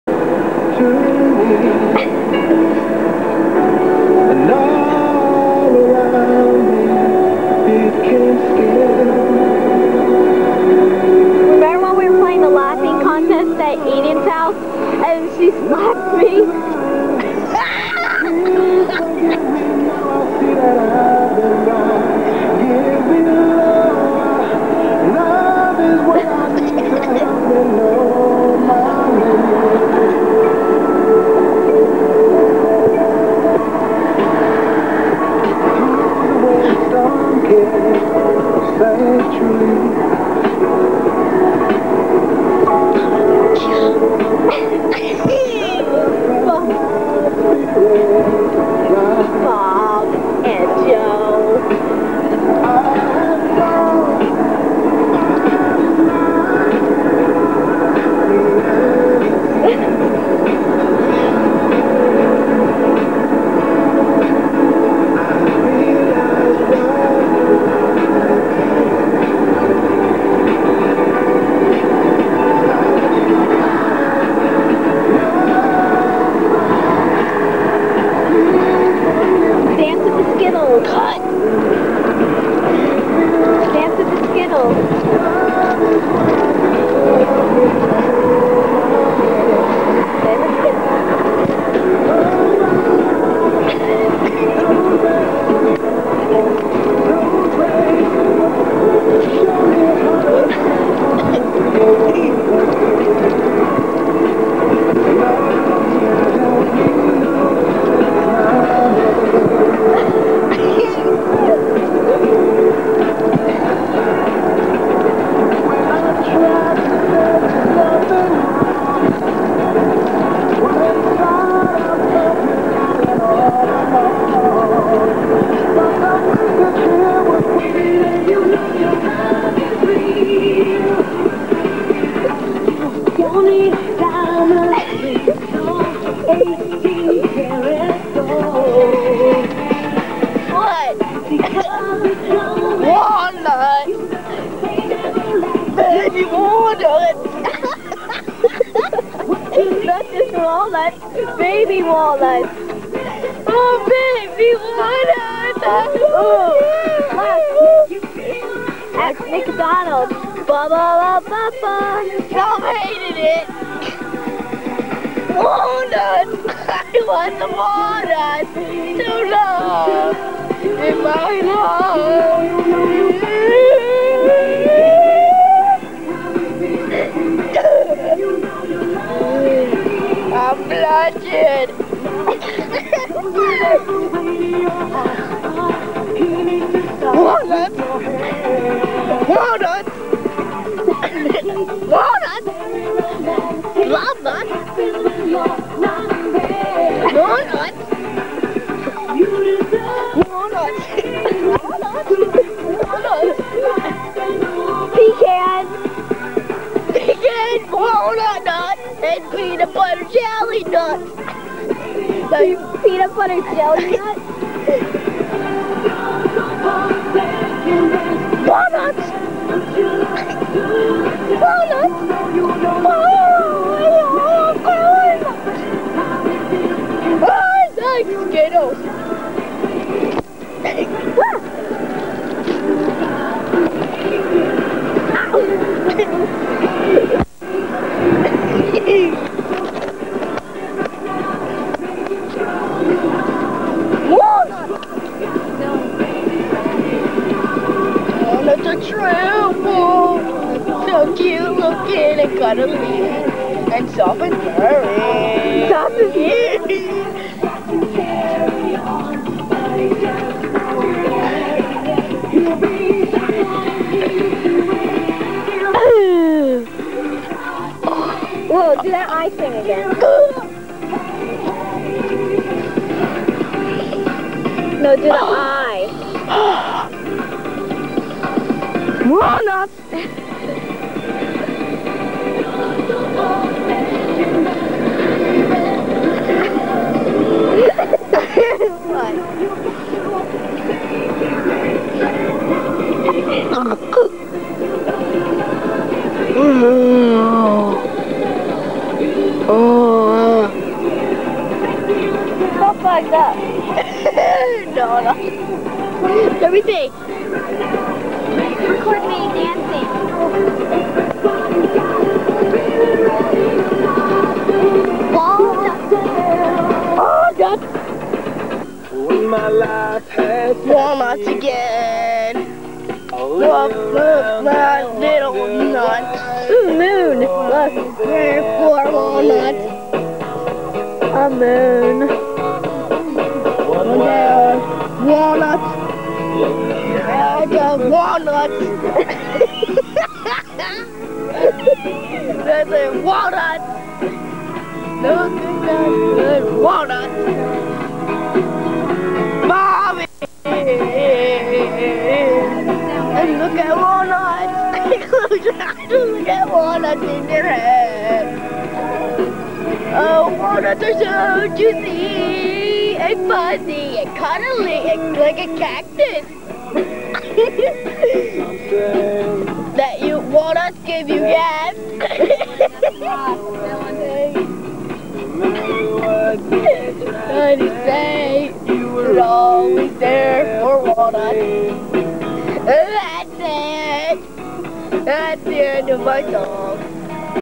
(NOT SO) AMERICAN IDOL: DYLAN
(2004 REWIND- Footage from The 2004 Adventures)

Dylan lip syncs and then makes up his own "walnut song".